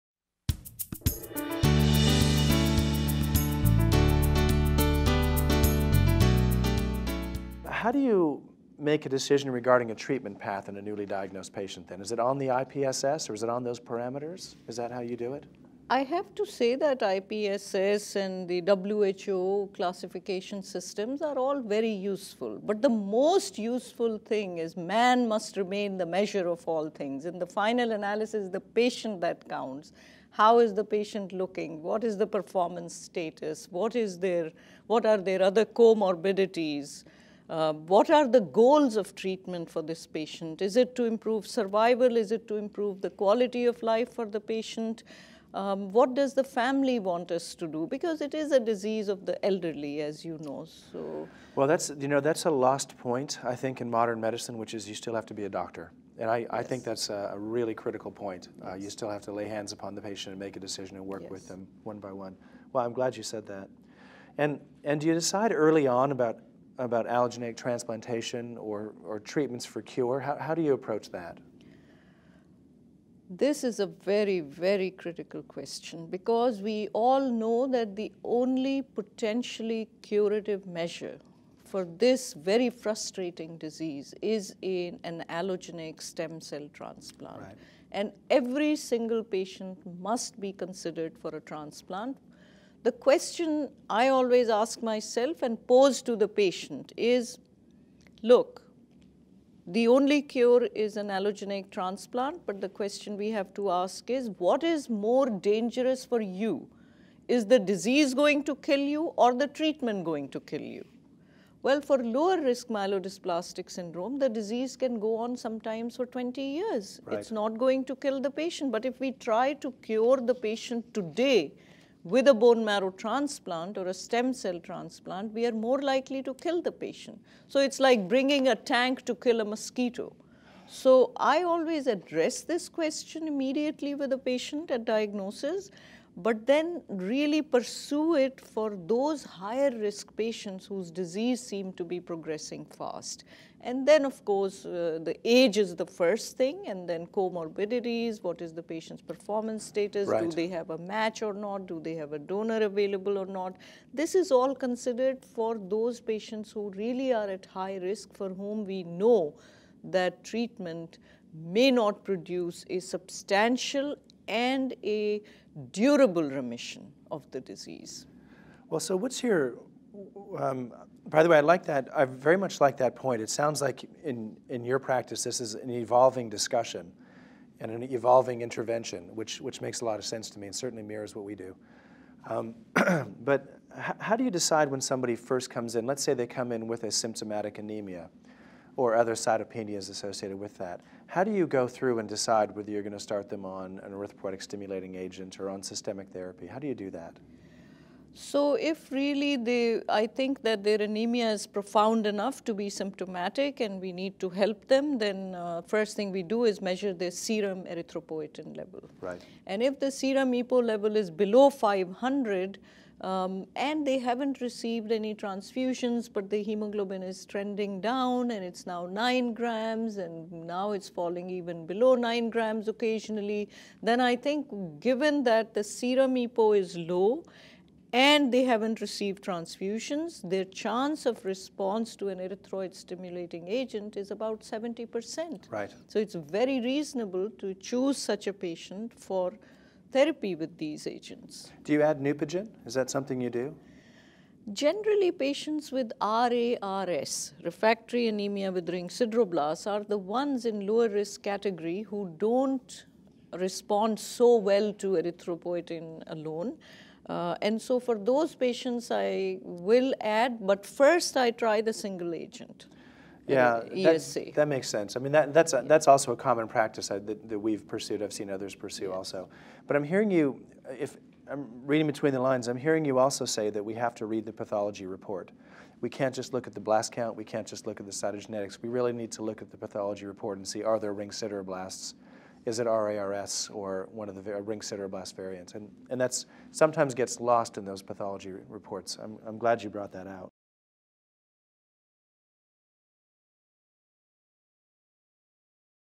MDS: Factors to Consider in the Initial Treatment Approach
James M. Foran, MD, FRCPC, and Azra Raza, MD, share how they make decisions with regard to what treatment path to take in a patient newly diagnosed with myelodysplastic syndrome.